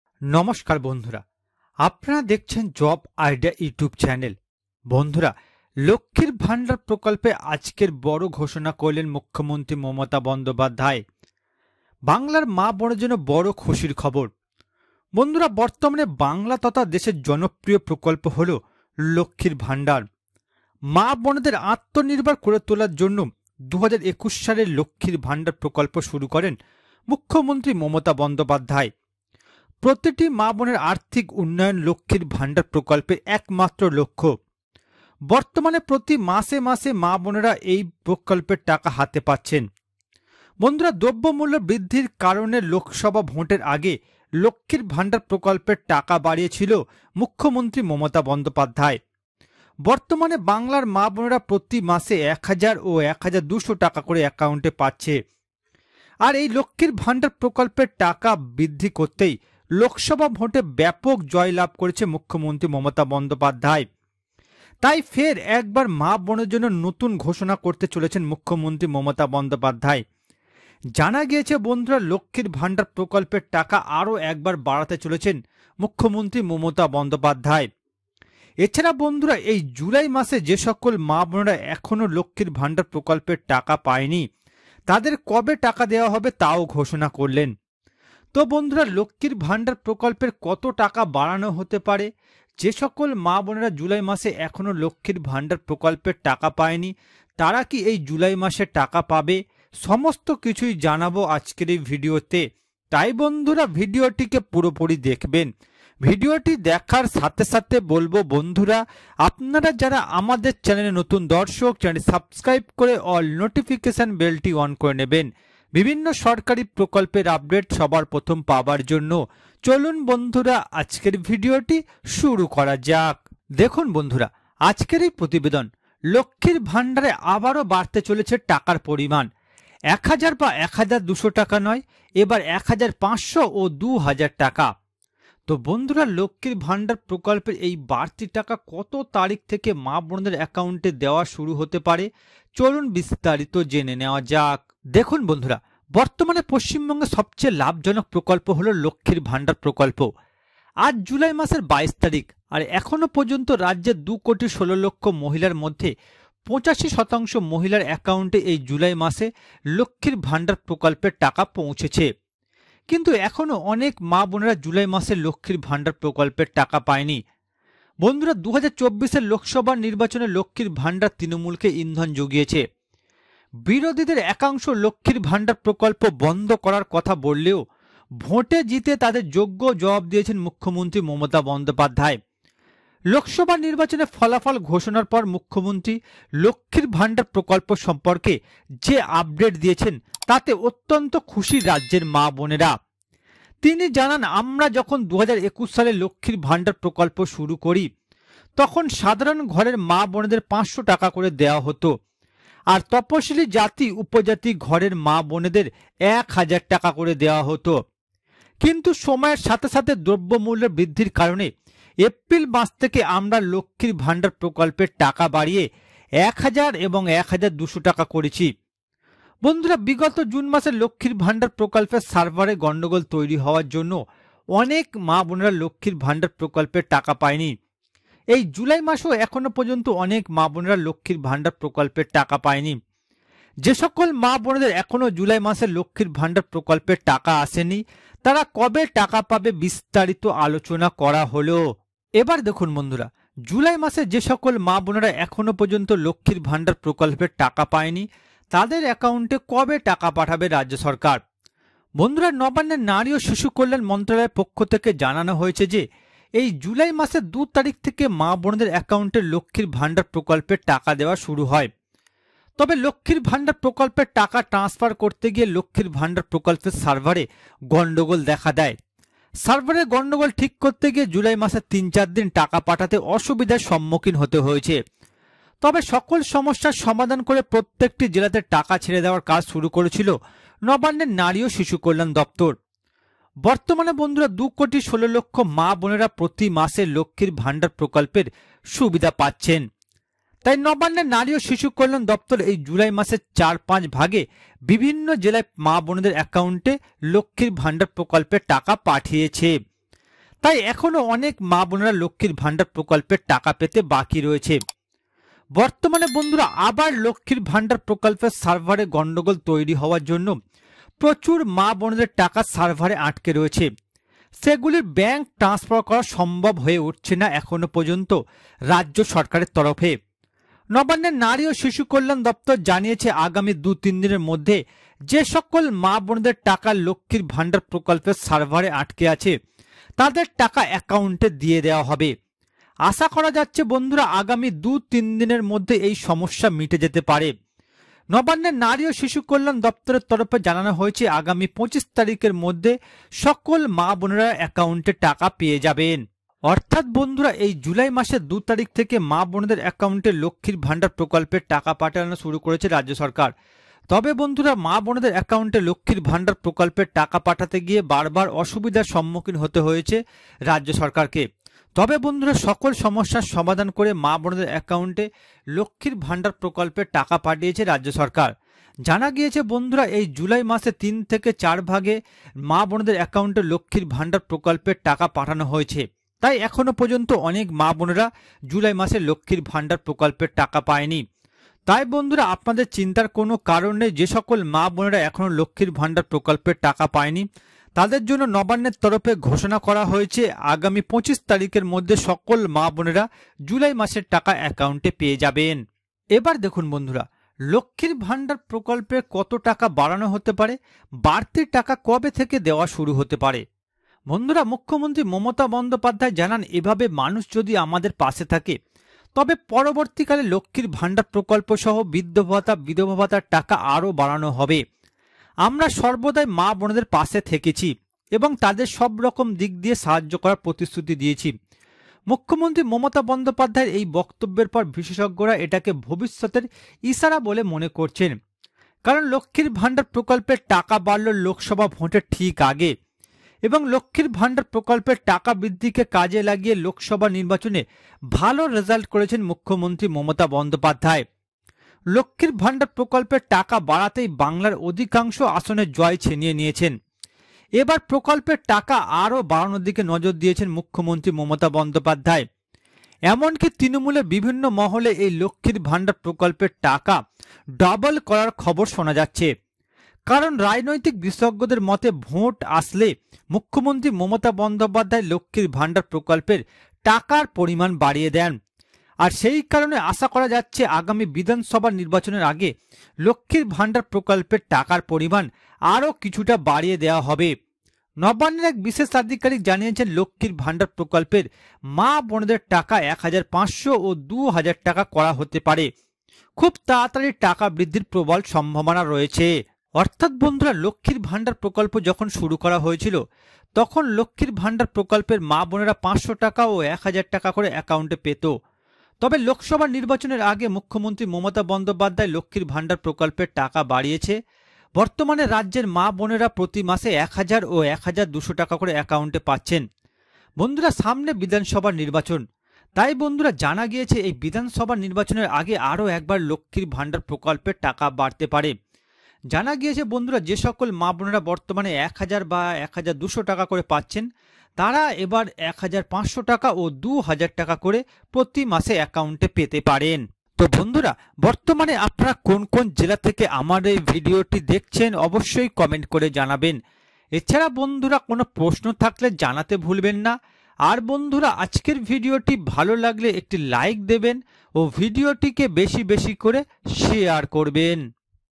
লক্ষ্মীর ভান্ডারে জুলাই মাসের টাকা কবে ঢুকবে? ডবল টাকা আসছে/Lokkhi Bhandar July Payment Date
লক্ষীর ভান্ডার
lokhir bhandar
lokkhi bhandar
lokhir bhandar